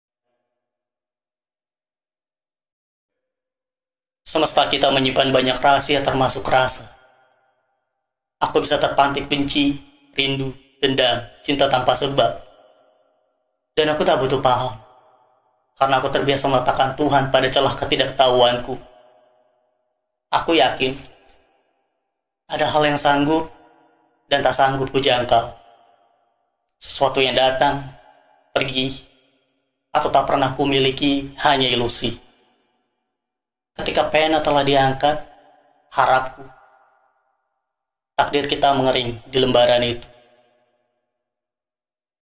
POEM - Aji : Semesta
Puisi berjudul "Semesta" adalah salah satu puisi karya Syahrian ( @syahriiaan ) yang dibacakan oleh Aji ( @fajririzqan )